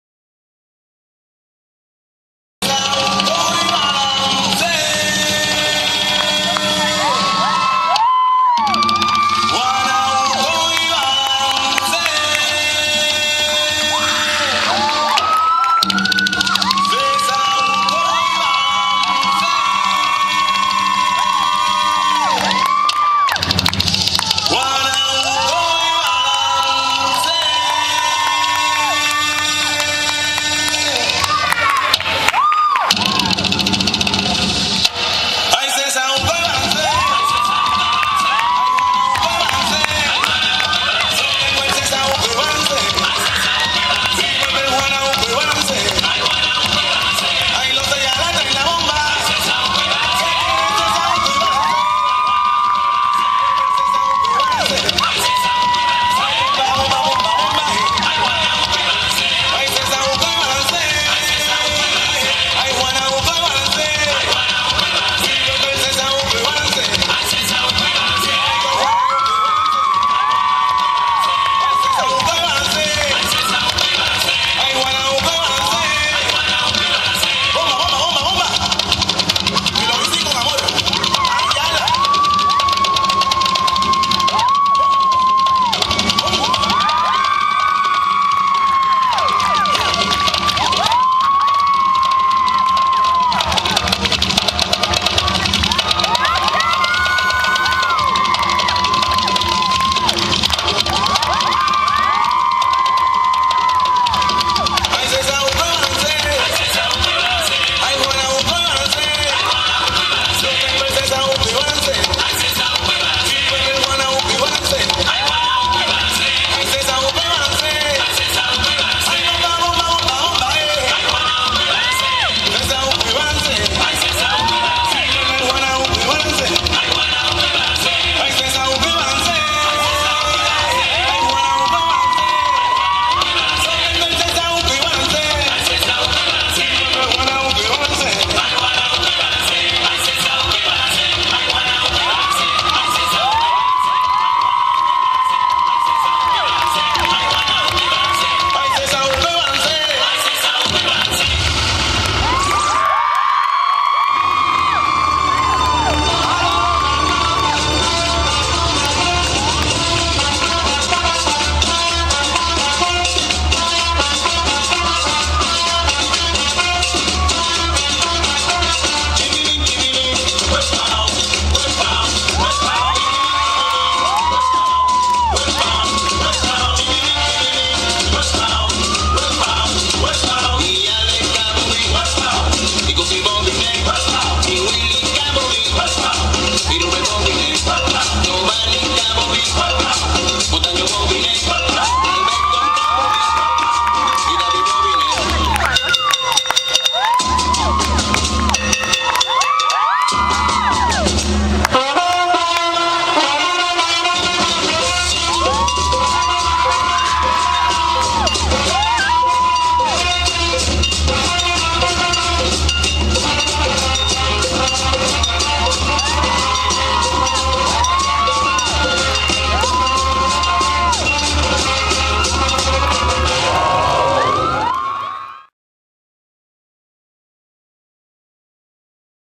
Gamma Phi Omega Bomba Performance at Latino Fest 08
Griselis
Ana
Celia
Gladys